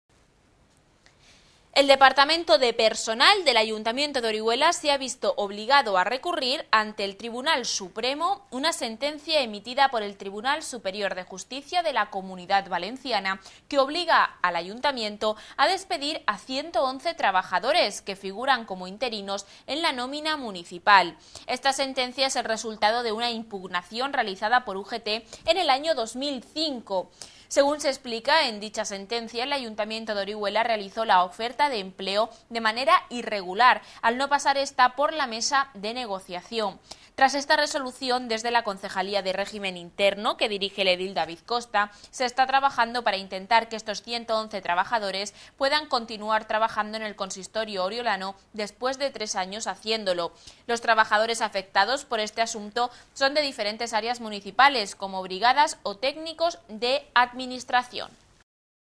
Ayto Orihuela 13-01-09 teleOrihuela
EL AYTO RECURRE UNA SENTENCIA QUE OBLIGA A DESPEDIR A 111 TRABAJADORES